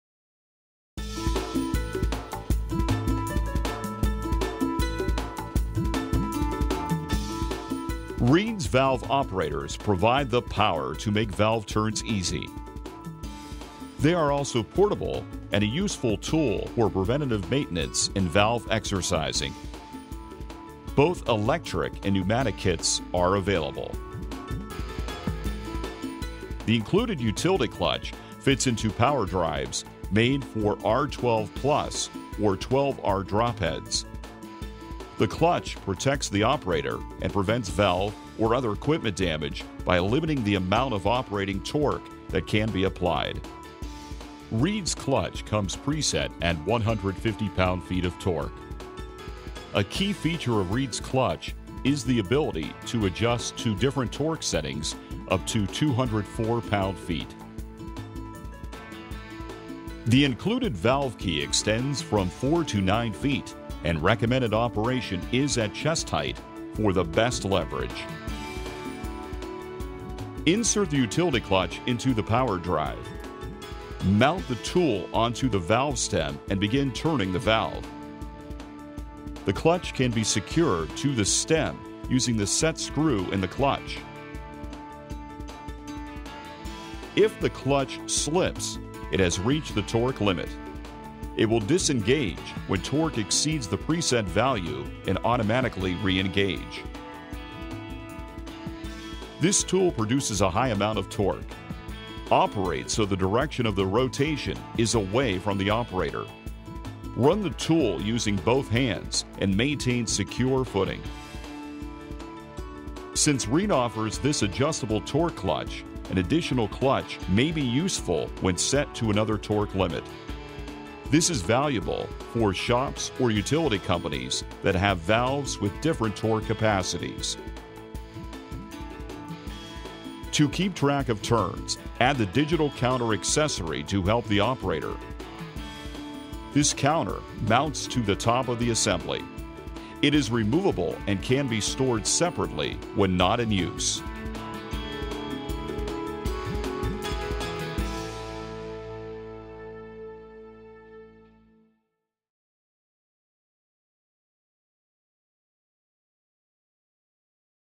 Portable Valve Operator for Valve Exercising Demo - Reed Manufacturing
REED’s Portable Valve Operators are inexpensive tools that are a great power alternative to hand turning valves.

Handheld Valve Operators provide power to make valve turns easy. This portable, power tool is great for preventive maintenance in valve exercising. Individuals working alone can isolate a leak by shutting multiple valves with this time-saving power tool. Electric (VOKU700PD) or pneumatic (VOKU700APD) power drive kits are offered. The utility clutch with over torque protection is adjustable but factory set at 150 lb-ft of torque. Clutch range is 7 to 204 lb-ft and disengages when torque exceeds preset value and automatically reengages. The valve key extends from 4 to 9 feet and recommended operation is at chest height for best leverage. One utility clutch (VOKUCLUTCH) ships with each kit. An additional clutch may be useful when set to another torque limit and valuable for shops or utility companies that have valves with different torque capacities. Add the Digital Counter accessory VOKCNT to keep track of turns for the operator

To learn more about REED, to browse all products, or find where to buy, visit our

Let’s Connect!